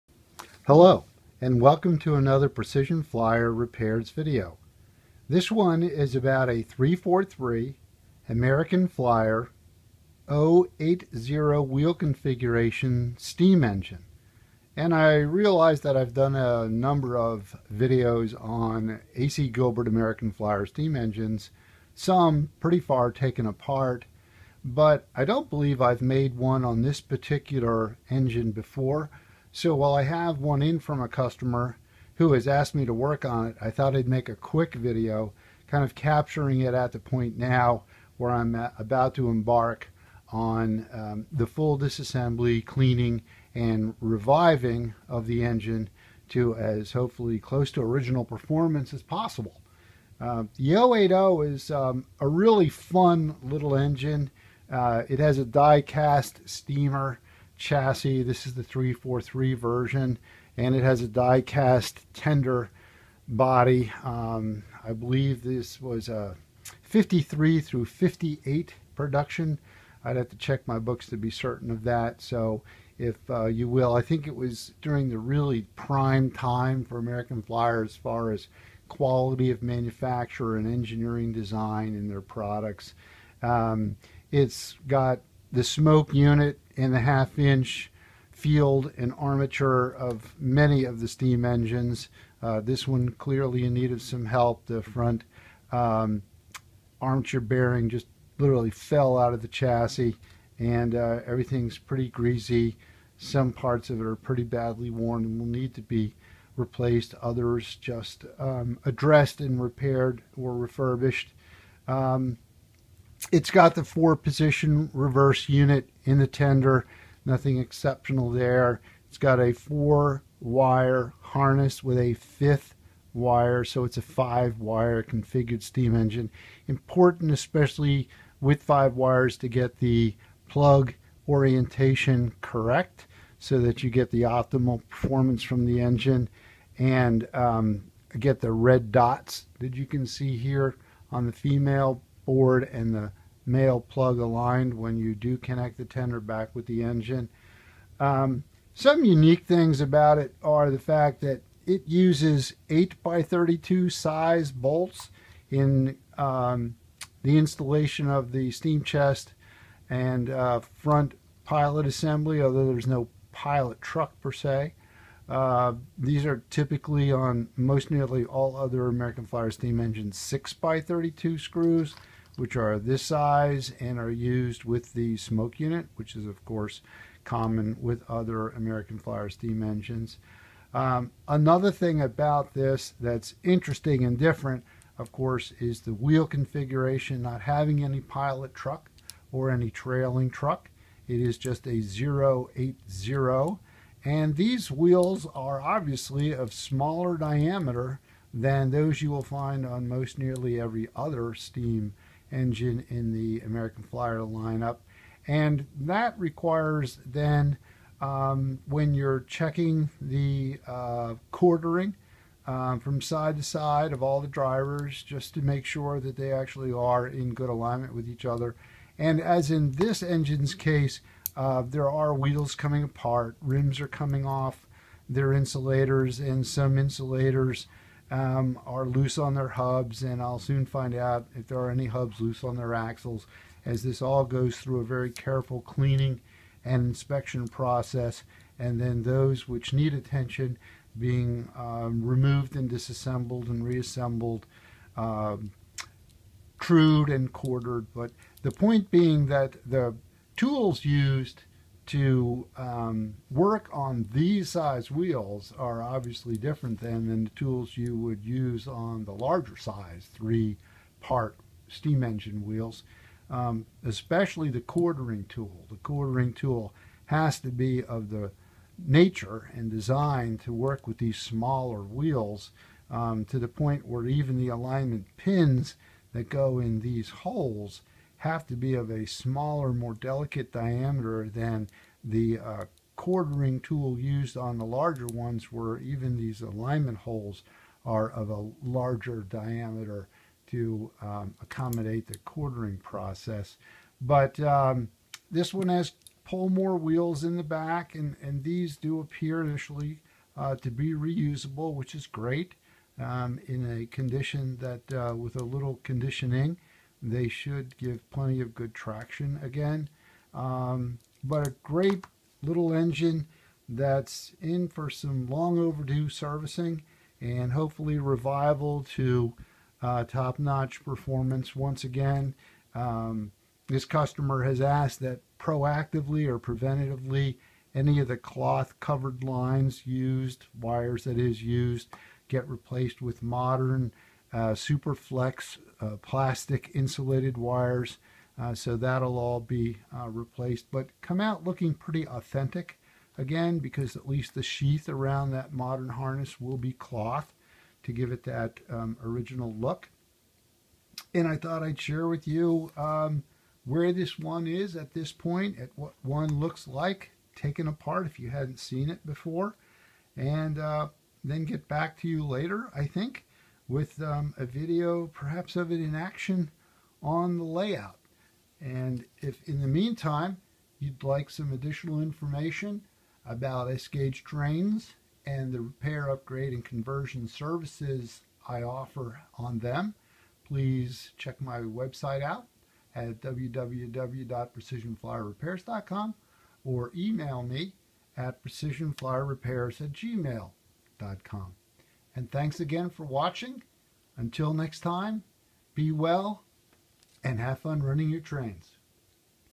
American Flyer 343 0-8-0 steam engine disassembled
This video shows an AF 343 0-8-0 disassembled prior to servicing.  For more information on S gauge train repairs, upgrades, and conversions Thank you. (Note: the boiler shell in this video is incorrectly referred to as chassis and "American Flyer Lines" is missing from both sides of the tender.)